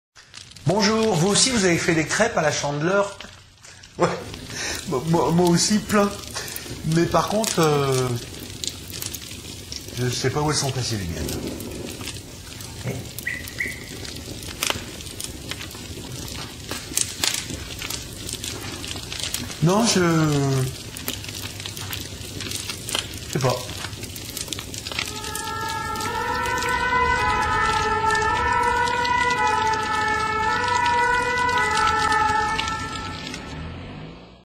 Mes putains de bonnes crêpes !!!
Vous avez fait des crêpes vous pour la Chandeleur ? Moi oui mais...